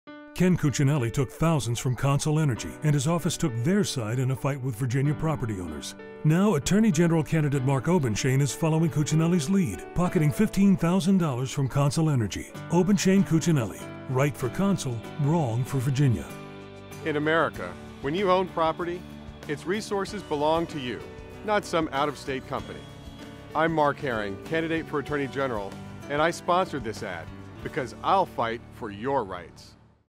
"Property"
Narrator: Ken Cuccinelli took thousands from Consol Energy, and his office took their side in a fight with Virginia property owners. Now Attorney General candidate Mark Obenshain is following Cuccinelli's lead, pocketing $15,000 from Consol Energy. Obenshain-Cuccinelli, right for Consol, wrong for Virginia.

Mark Herring: In America, when you own property, its resources belong to you—not some out of state company.  I'm Mark Herring, candidate for Attorney General and I sponsored this ad because I'll fight for your rights.